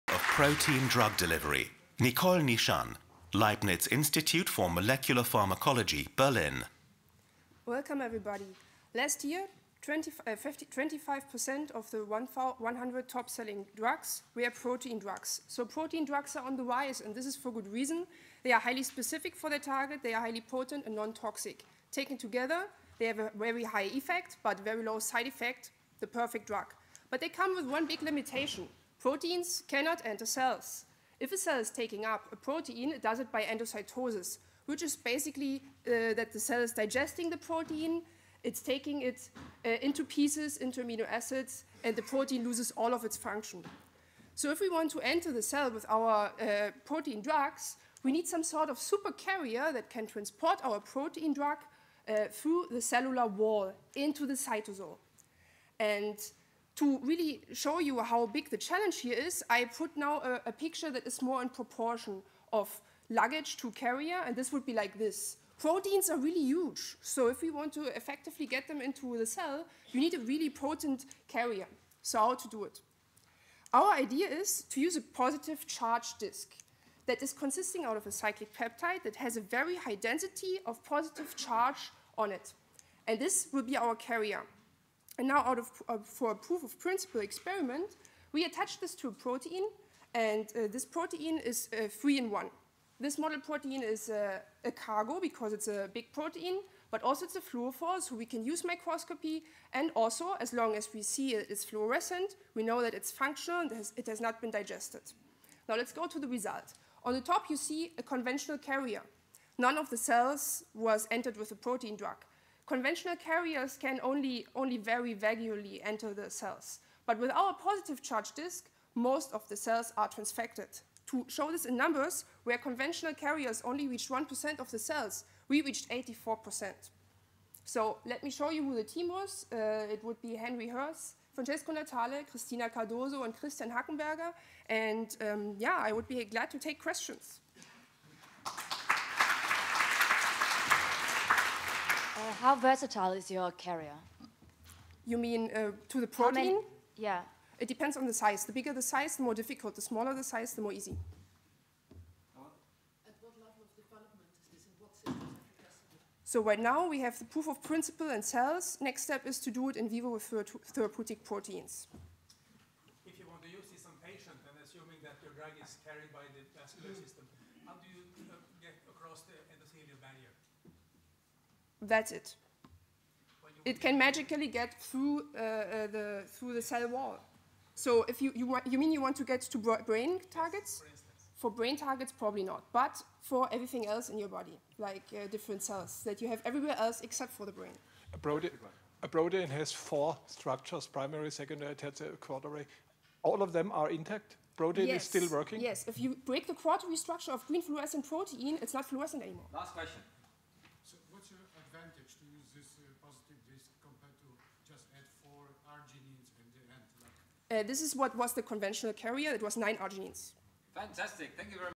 Nicole Nischan - Breaking the wall of protein drug delivery (Falling Walls 2014)
Copyright: Falling Walls Foundation